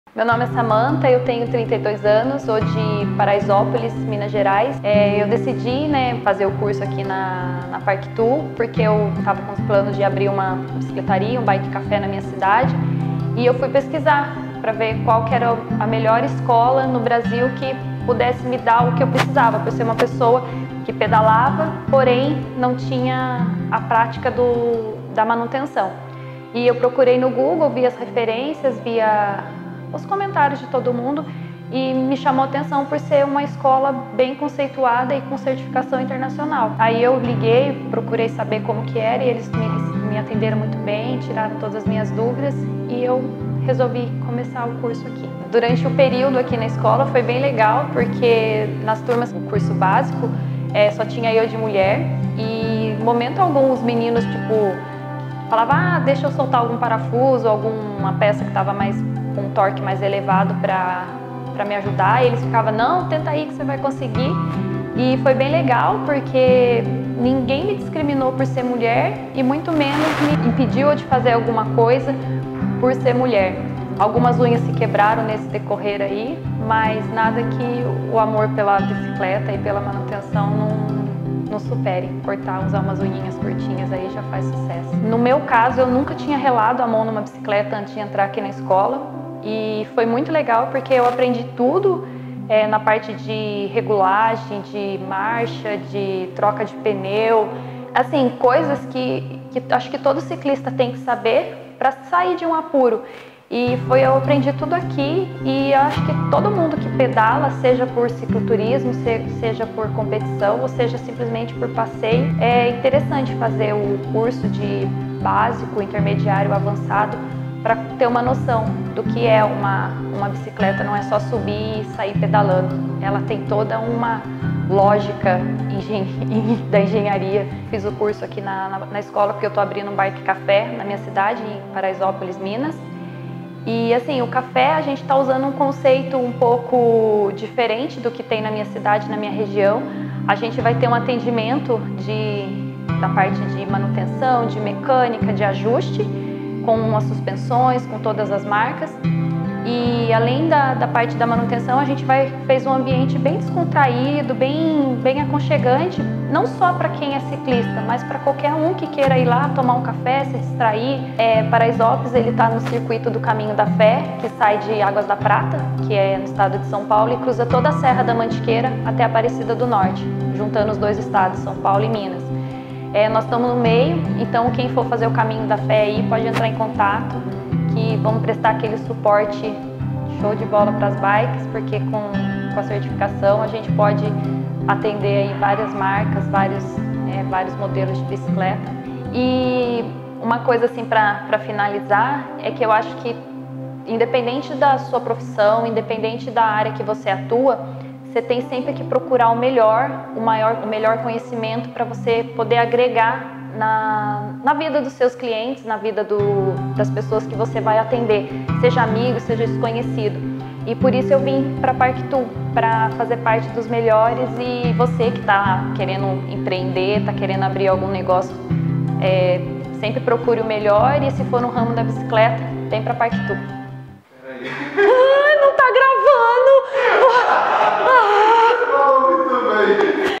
Mulheres na Mecânica: Conheça História de Samanta Ribeiro
Conheça agora a história da Samanta que veio diretamente de Minas fazer curso conosco!
Histórias que nos fazem continuar, nos inspiram a continuar tornar sonhos em realidade.
Parabéns Samanta! Conte sempre com a escola Park Tool!
Venha você que está assistindo esse video fazer parte da melhor escola de mecânicos de bicicletas do Brasil!